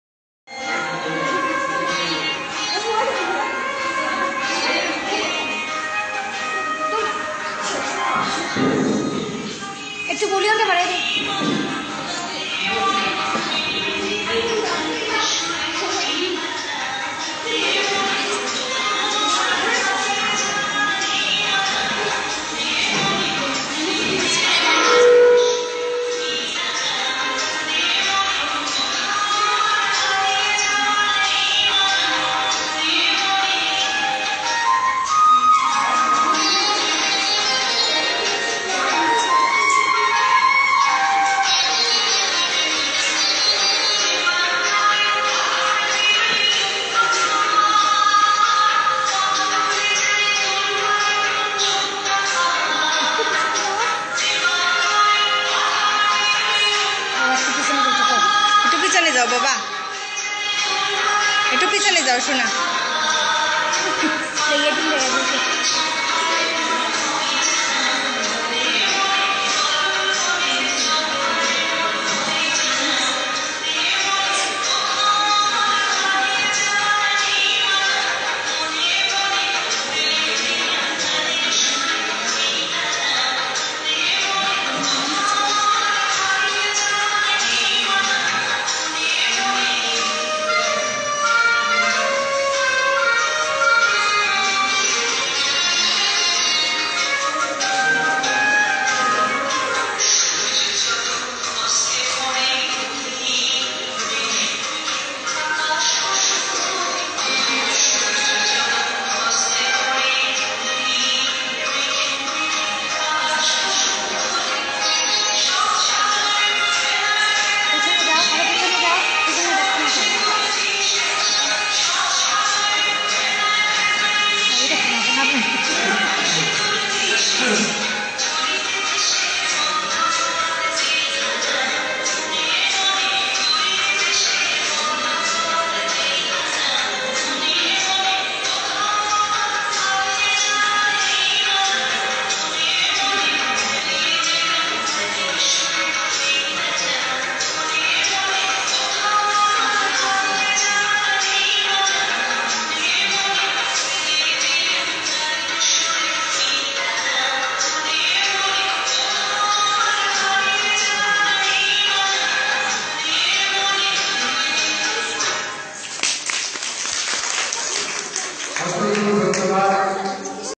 Mithila's Dance Video 1
Mithila Rani Nath Juthi is a 4 year old girl living in Dharmanagar, North Tripura, India. She loves singing , dancing and fighting. This is her first  dance video at Dharmanagar Public library Hall on 10 August, 2015.
She wants to be a great performer. She badly requires all viewers´ kind inspiration.